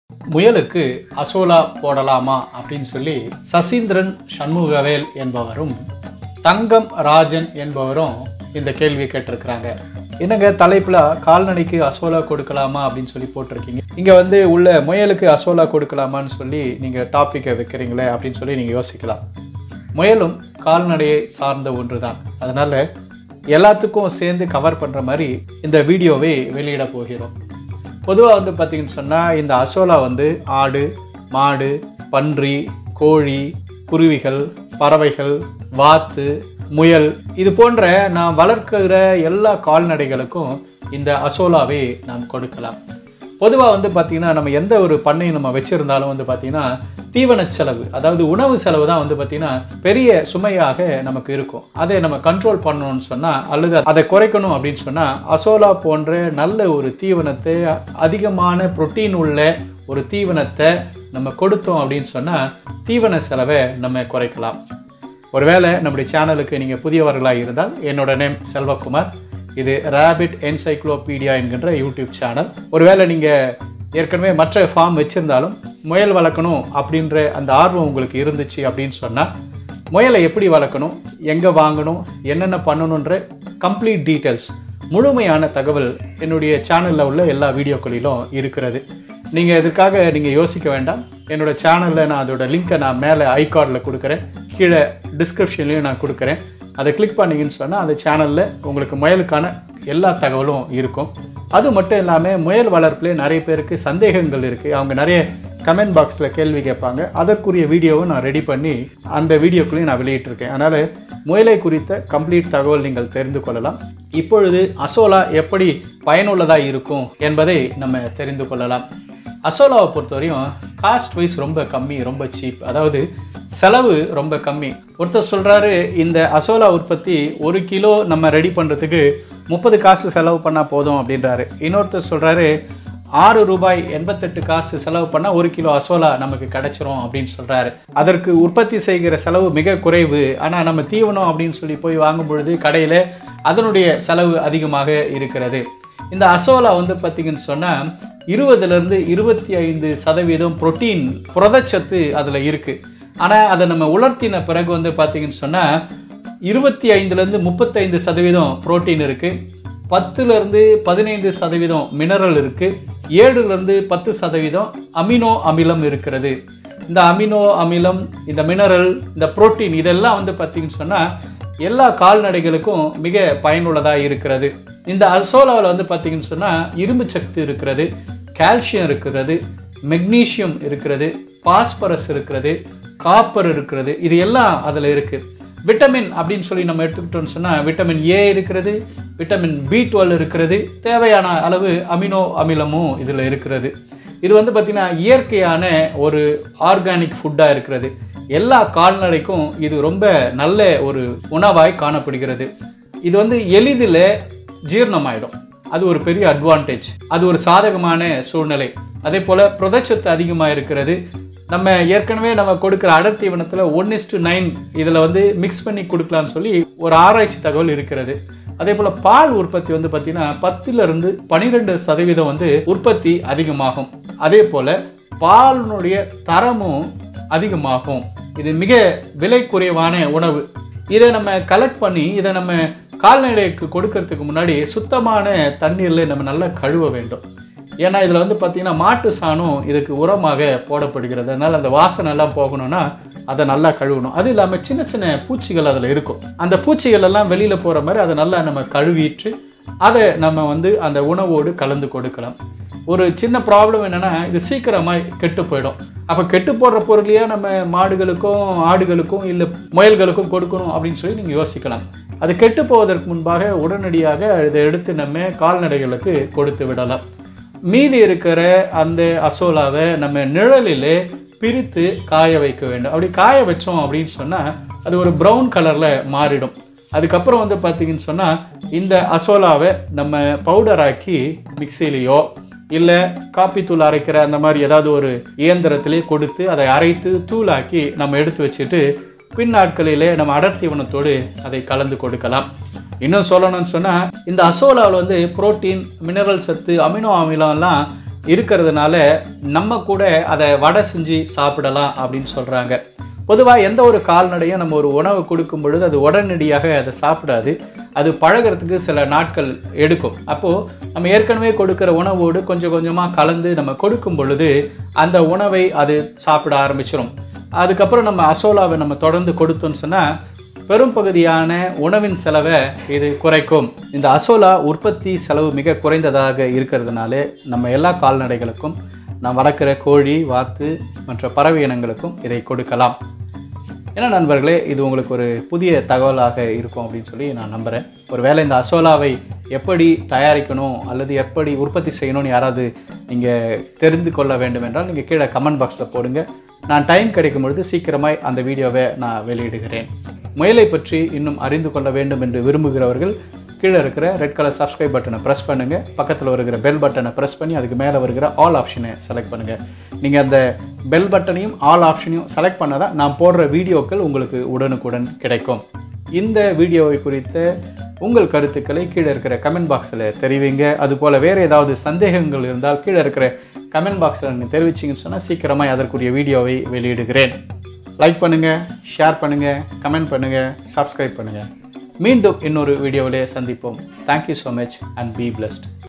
கால்நடைகளுக்கு அசோலா கொடுக்கலாமா? | அசோலா தீவன செலவை குறைக்குமா? | Rabbit Encyclipedia | Muyal Valar
கால்நடைகளுக்கு அசோலா கொடுக்கலாமா? | அசோலா தீவன செலவை குறைக்குமா? | Rabbit Encyclipedia | Muyal Valarpu